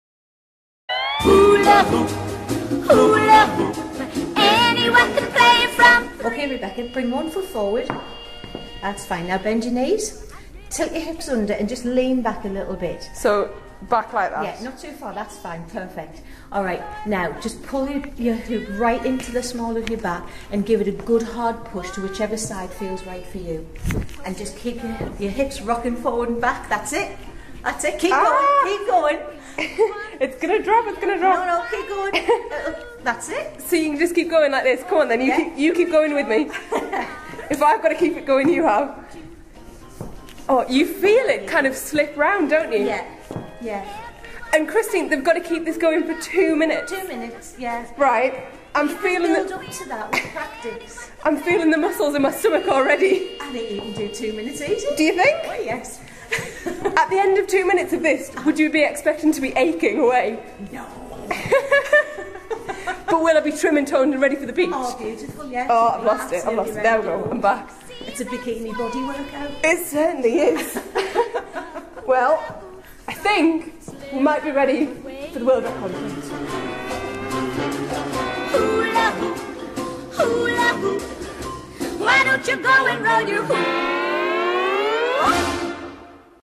Hula Hooping
Metro Radio's Rebecca Dixon having her first go at learning to hula hoop since she was a child, ahead of the Sunderland World Record attempt on June 16th 2012